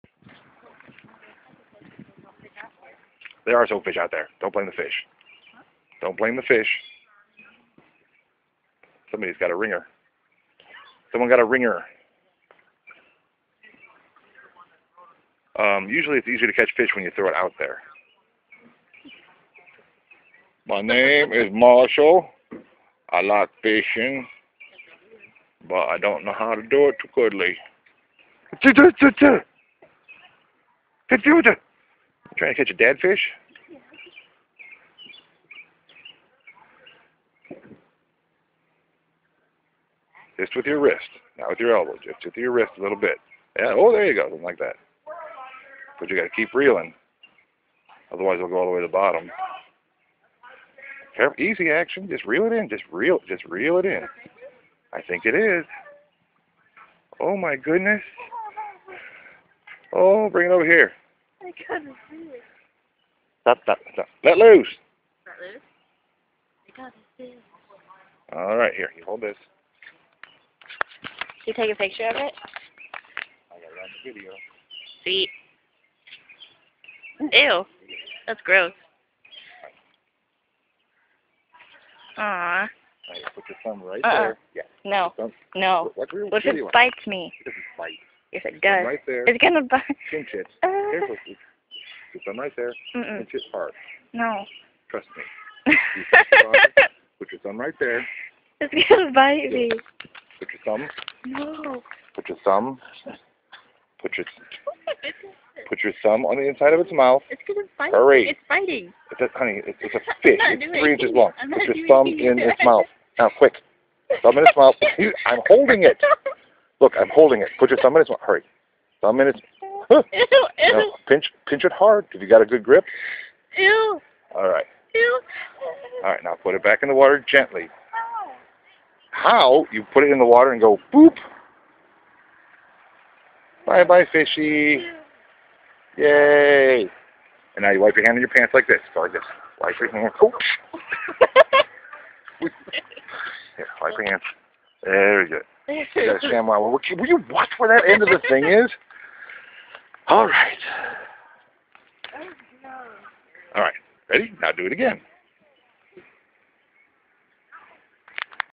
Moo fish
Marshall catches a fish and releases it...eventually.